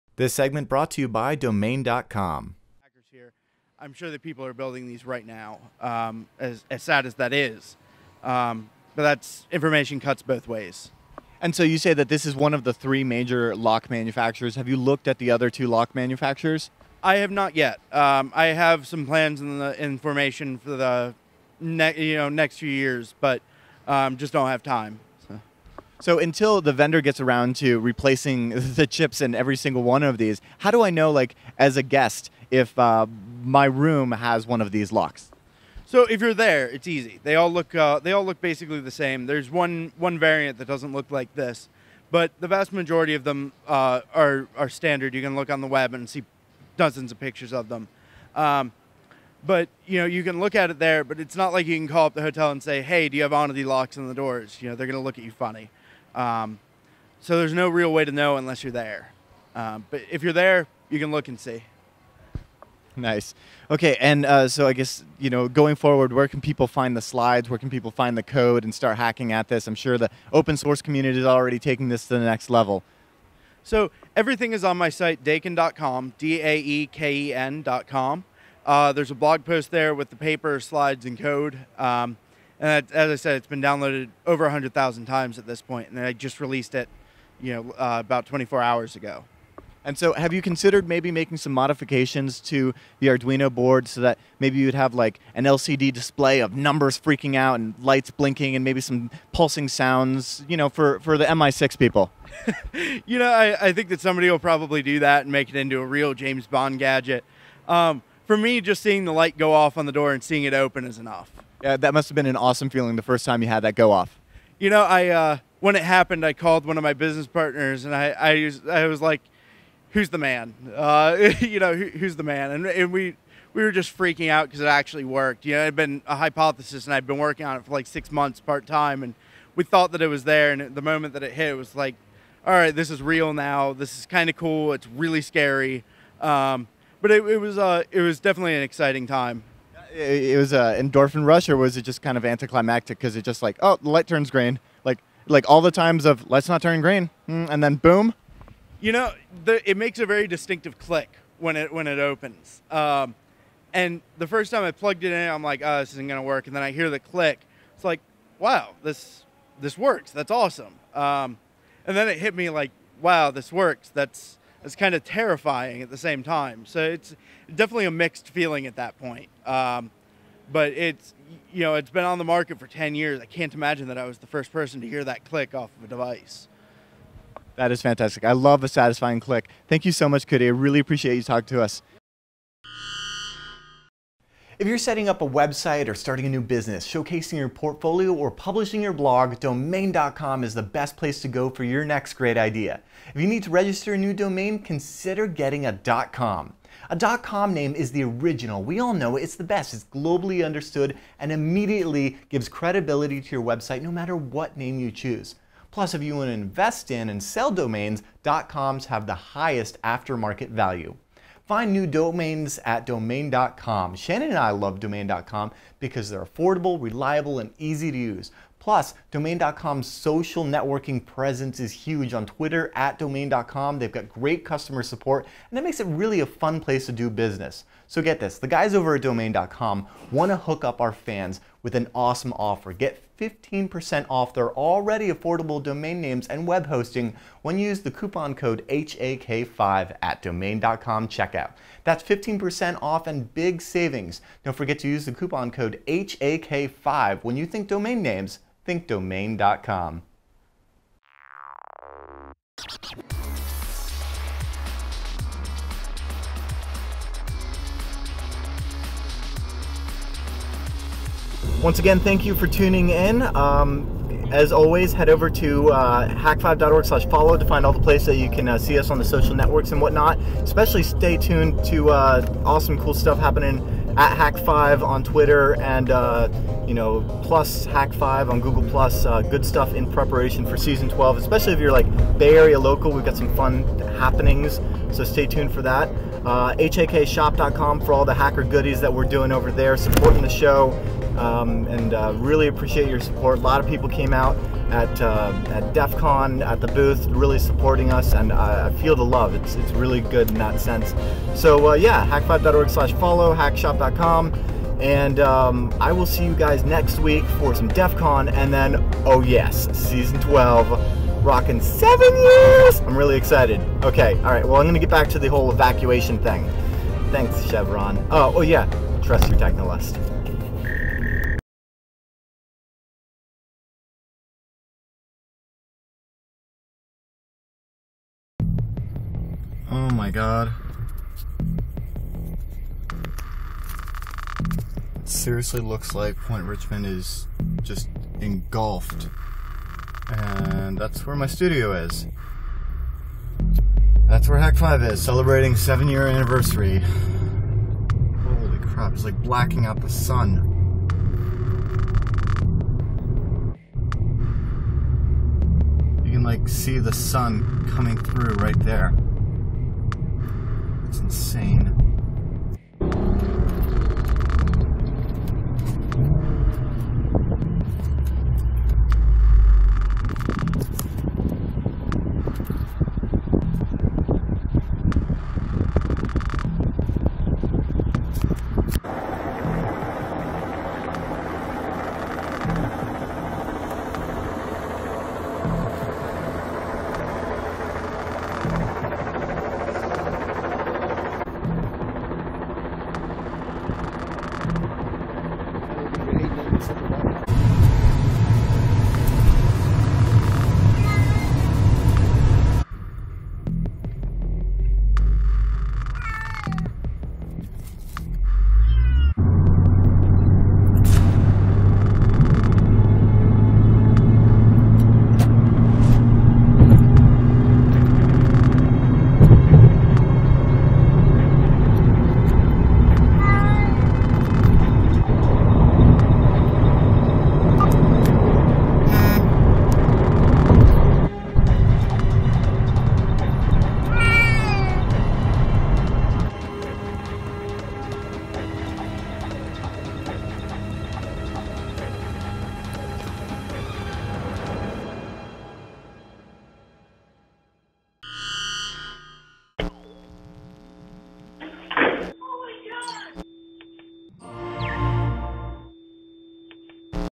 Hak5 - Black Hat 2012 - Cracking firmware and physical locks, Hak5 1125.3
Hak5 -- Cyber Security Education, Inspiration, News & Community since 2005:
This time in the show from Black Hat 2012, popping hotel key card locks and soho wifi routers with elegance. All that and more on this special episode of Hak5.